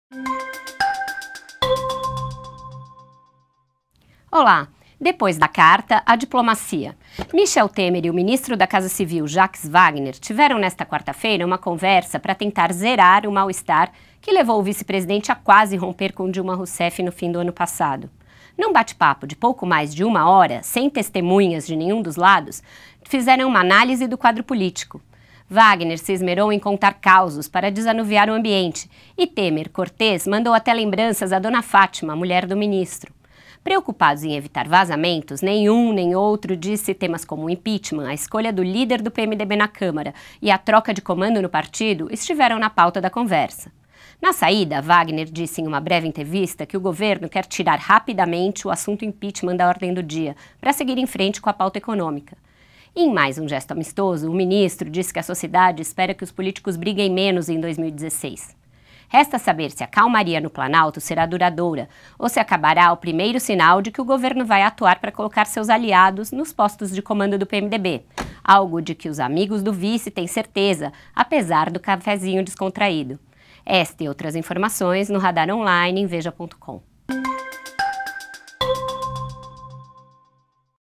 Temer e Wagner têm conversa diplomática
Chefe da Casa Civil tenta contornar mal-estar com vice-presidente. Acompanhe no comentário da colunista de Radar On-line Vera Magalhães.